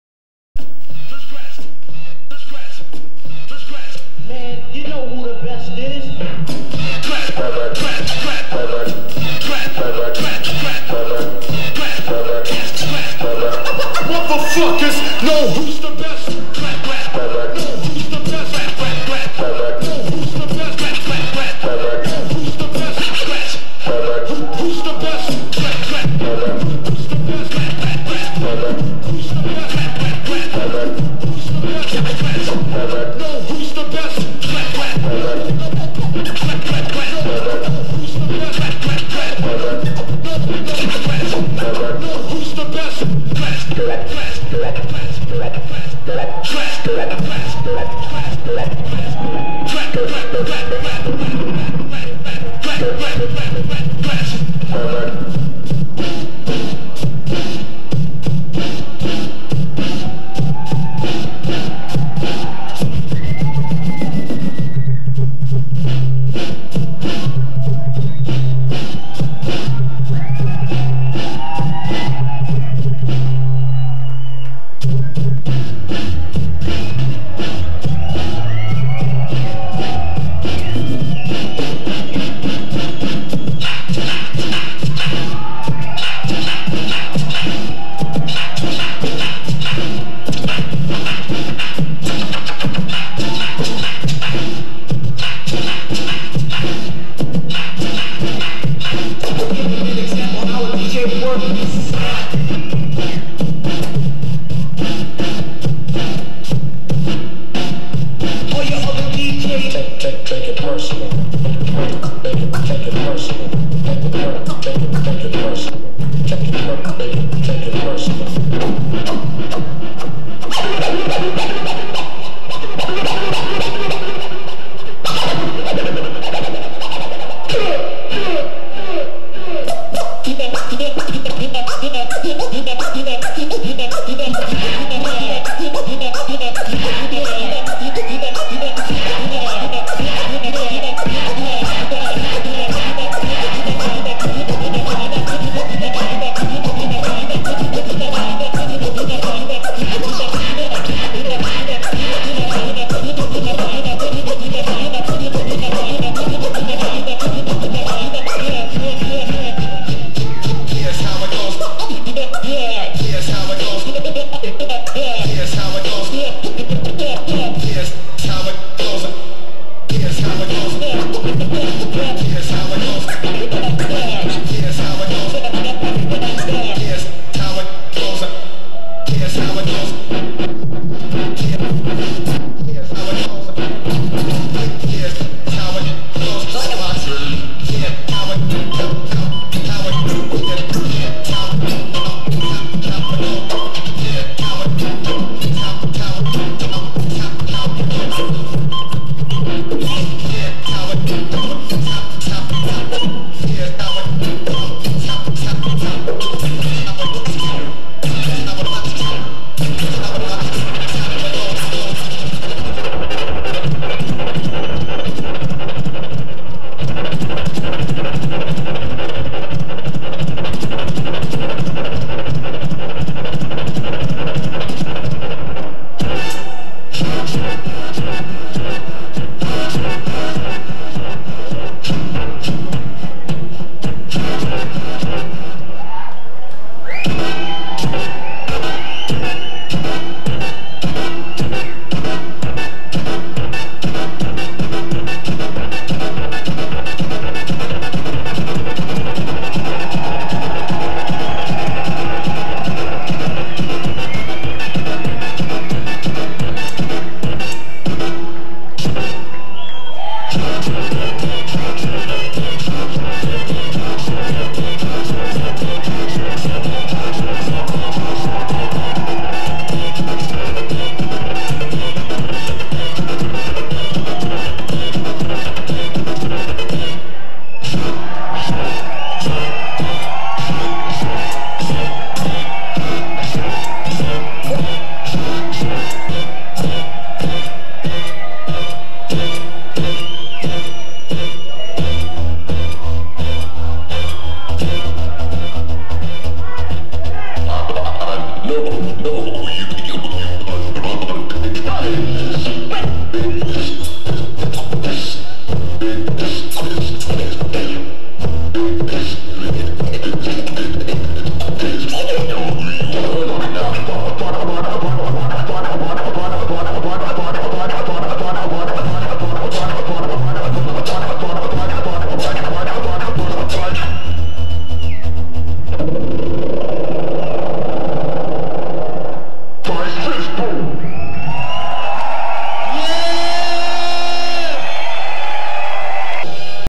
Scratch Perverts — 1998 ITF Eastern Hemisphere Team Finals (Champion)
"You know who the best is!" By subscriber request, the winning performance of UK's greatest team the Scratch Perverts at the 1998 ITF Eastern Hemisphere Team Finals, Hamburg, Germany, featuring Tony Vegas, Prime Cuts, First Rate, and Mr. Thing. The Scratch Perverts would go on to the World Final in Amsterdam, where they were narrowly defeated by the defending champion Beat Junkies. Footage courtesy of Julien Regiero.